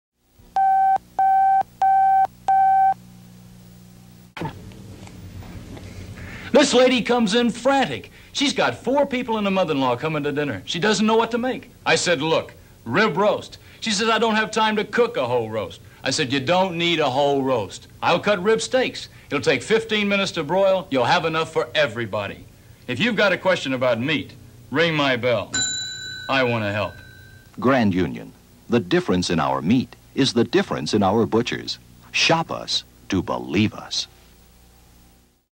The Butcher Rings the Bell.mp4
Early Grand Union Family Markets commercial "The Butcher Rings the Bell.  Filmed during Gil Gerard's early days in NY